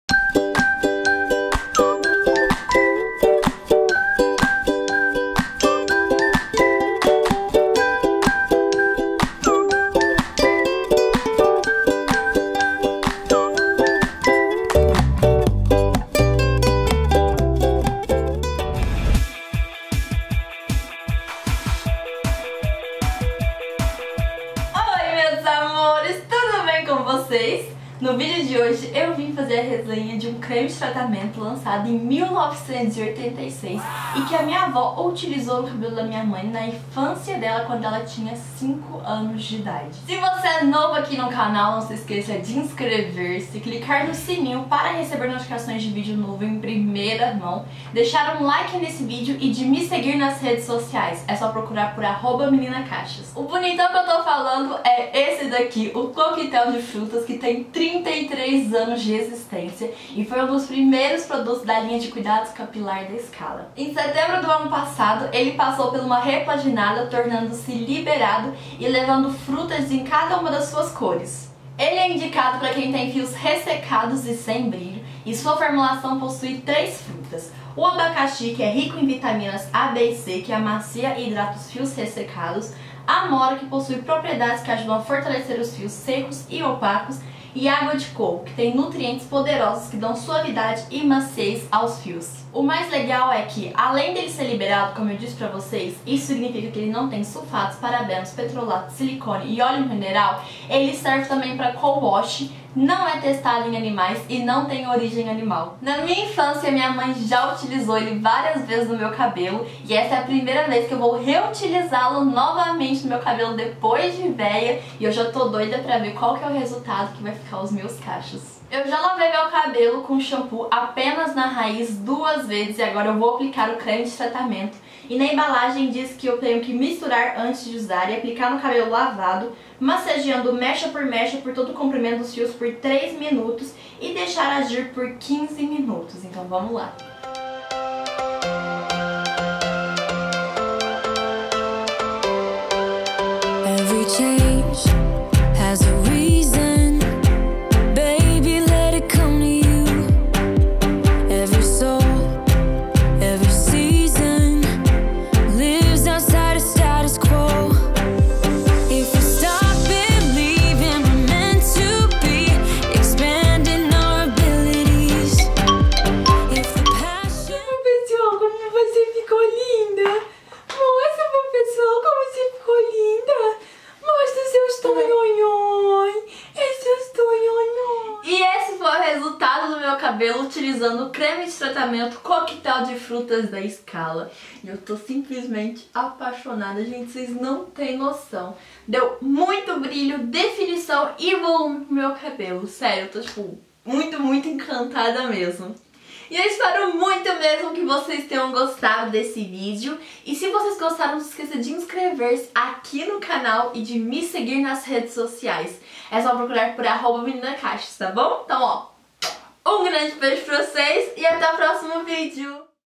TESTEI UM CREME DE HIDRATAÇÃO DE 1986!
Oi meus amores! Tudo bem com vocês? No vídeo de hoje eu trouxe a resenha do creme de tratamento Coquetel de Frutas da Skala! Você já usou?

Espero que tenham gostado do vídeo! Assista em HD!

Maria Eduarda Guerra
Caixa Postal 181
CEP 38001970
Uberaba - MG